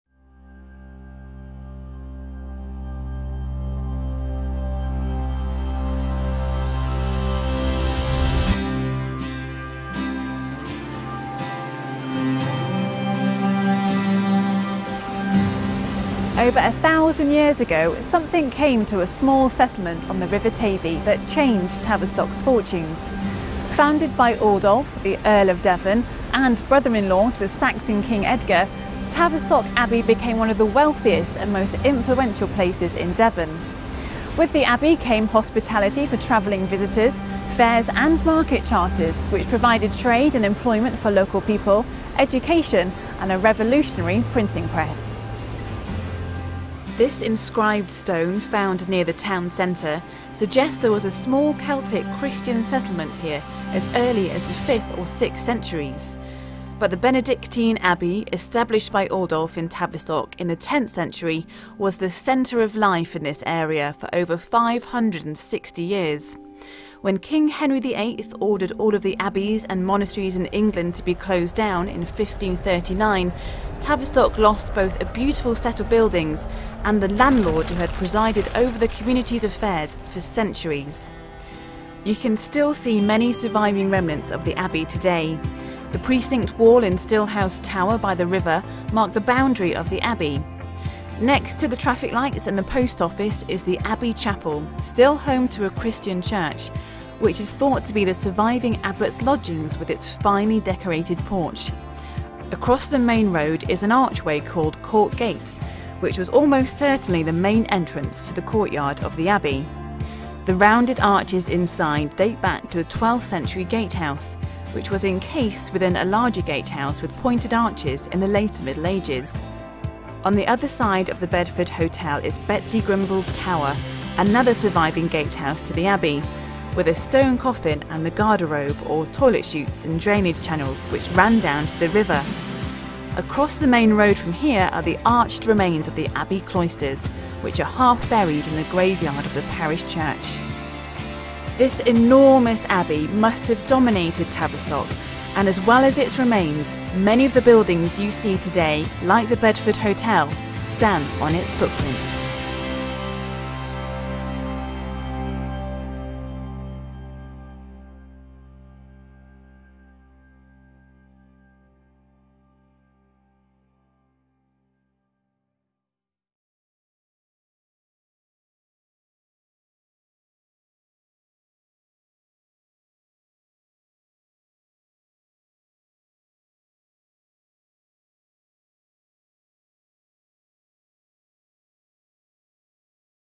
Tavistock Heritage Films - 1 Tavistock Abbey
Tavistock Abbey
Welcome to the Foundations of Tavistock.

Over 1,000 years ago, something came to a small settlement on the River Tavy that changed Tavistock's fortunes forever...
Watch the film to find out more about the foundations of this bustling market town with independent shops, cafes, pubs and restaurants that share this common Heritage.

Tavistock is a Unesco World Heritage Site

These films were funded by Tavistock Business Improvement District Ltd.

These films were created by Fatcalf Media.